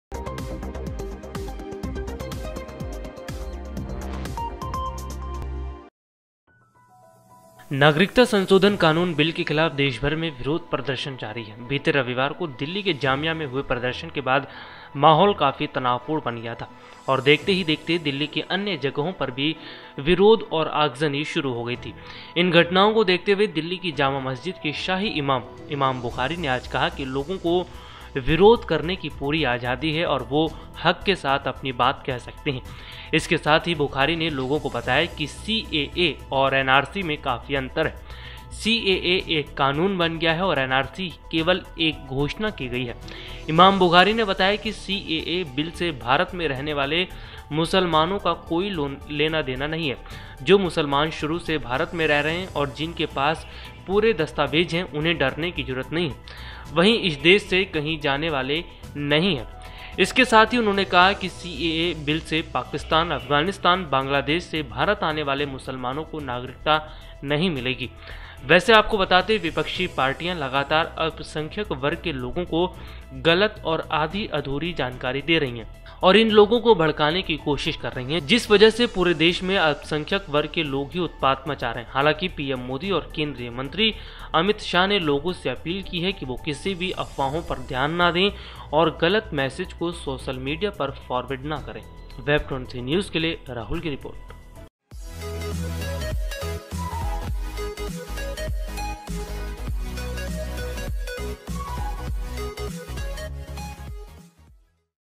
नागरिकता कानून के विरोध को देखते हुए जामा मस्जिद के शाही इमाम का बड़ा ऐलान…
नागरिकता संसोधन कानून बिल के खिलाफ देशभर मैं विरोध प्रदर्शन जारी है बीते रविवार को दिल्ली के जामिया मैं हुए प्रदर्शन के बाद माहौल काफी तनावपूर्ण बन गया .

Follow us on twiter @web23N